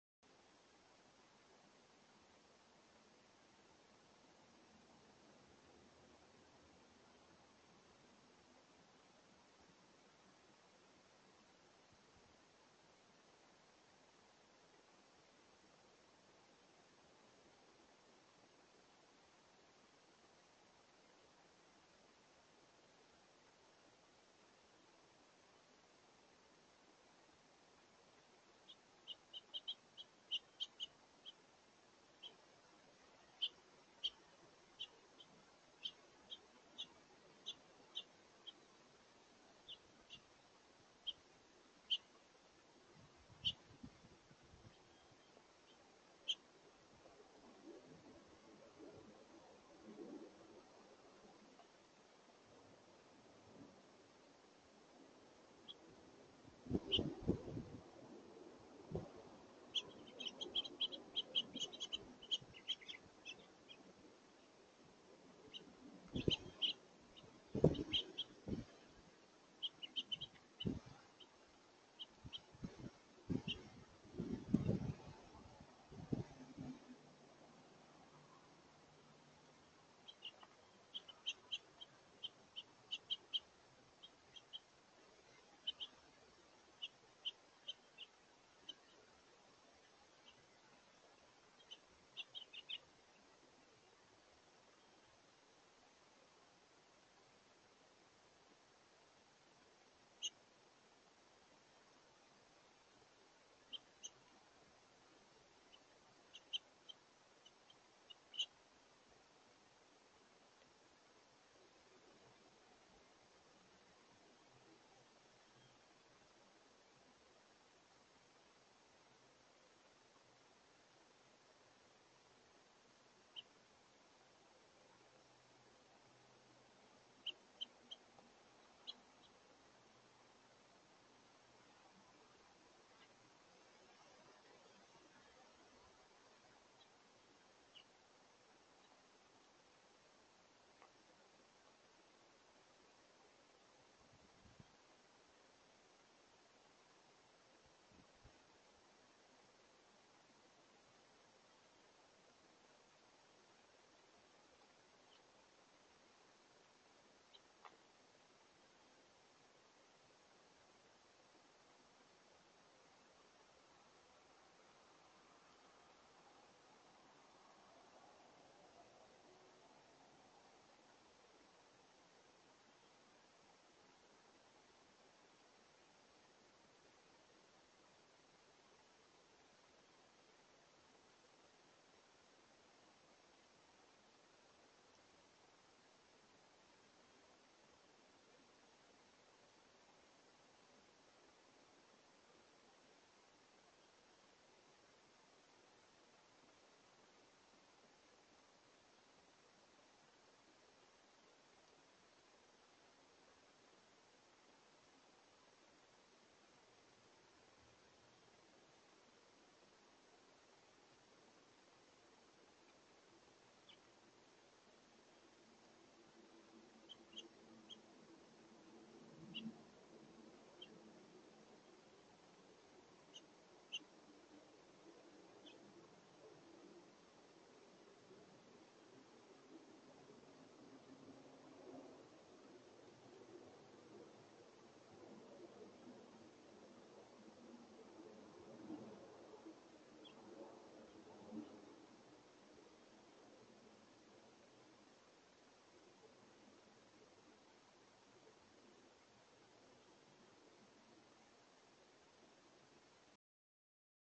2023/11/16 - Several House Finches fly in for a brunch time meal on a warm windy day
These House Finches time things well.  Shortly after this the winds pick up even more.  Hopefully you enjoyed the video.